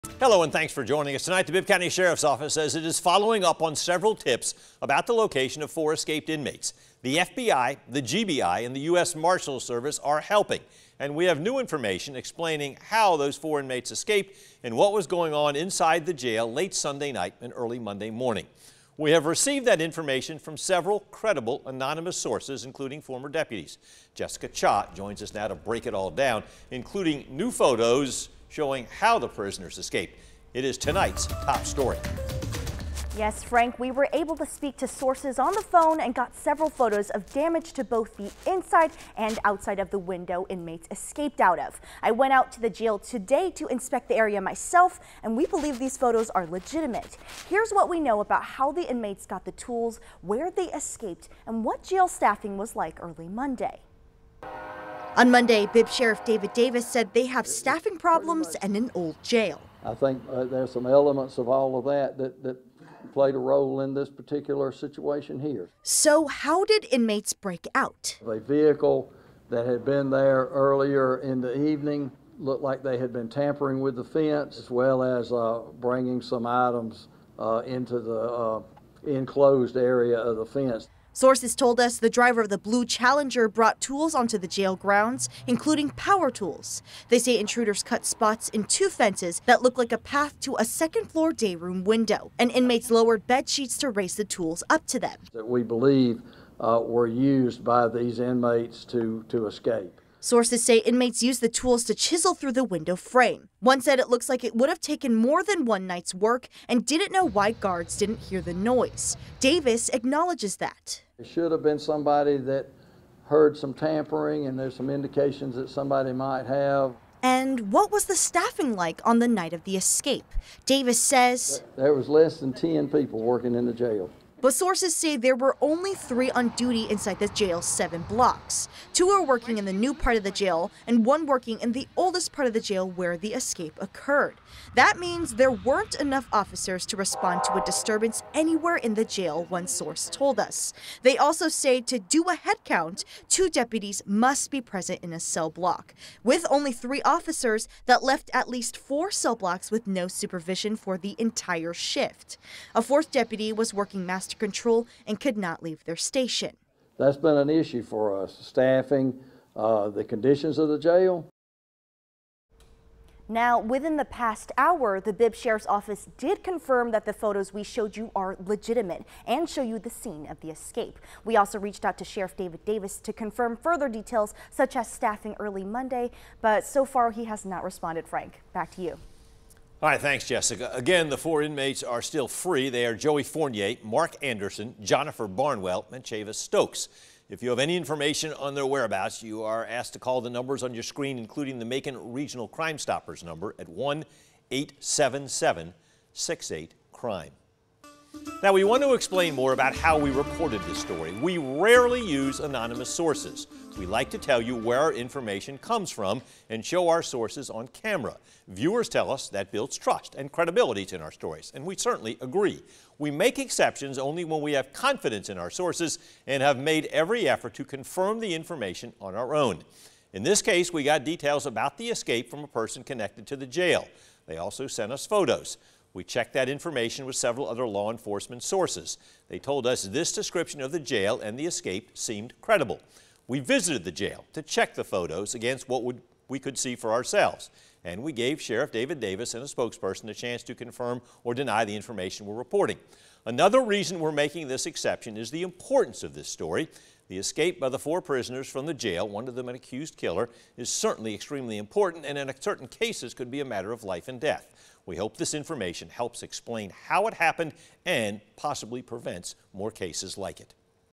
Inside the escape: Leaked photos show how 4 inmates escaped Ga. jail
More details are coming out about the escape earlier this week at the Bibb County jail, including the staffing during the shift and how the inmates may have escaped.

13WMAZ spoke to several anonymous sources with connections to the jail, including former deputies. They provided photos and information on the jailbreak and what may have happened on Monday morning.

According to sources, a blue Dodge Challenger brought tools to the jail grounds, including power tools that the inmates used to chisel through the window to create enough space for them to escape.

People who saw the damage to the windows say it may have taken more than one night to chisel through.

At this time, 13WMAZ is working to confirm reports on how the inmates received the tools.

One source said that a drone was used to bring the chisel over, and another says that intruders cut through two fences, inmates lowered bedsheets and raised the tools to the second floor.

Multiple sources told us they are not sure how no one inside the jail heard this going on.

"There should've been somebody that heard some tampering, and there's some indications that somebody might have," Sheriff David Davis said during his news conference on Monday.